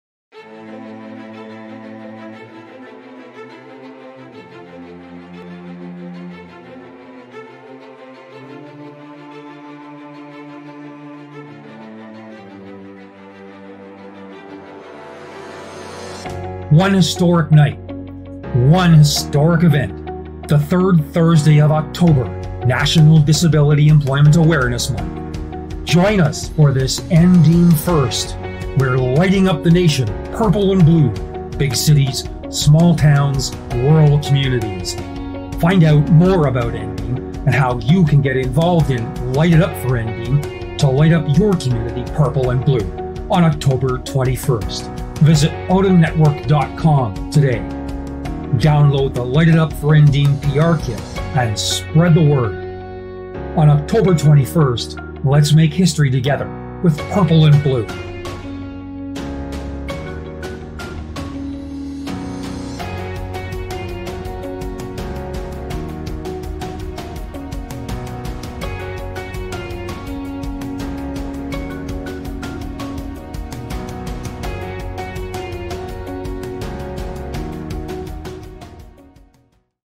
Light It Up! For NDEAM - October 21, 2021
ODEN, and the Canadian Association for supported Employment (CASE) and MentorAbility Canada, are collaborating on the first-ever, national Light It Up! For NDEAMTM event during National Disability Employment Awareness Month (NDEAM) in October 2021.

Light It Up! For NDEAM spotlights the many ways people who have a disability contribute to businesses and their communities, helping companies be successful and competitive.

Watch the video to learn more, and find out how to participate in lighting up the nation purple and blue in communities across Canada, on October 21.

(Light It Up! For NDEAMTM is a trademark of the Ontario Disability Employment Network.)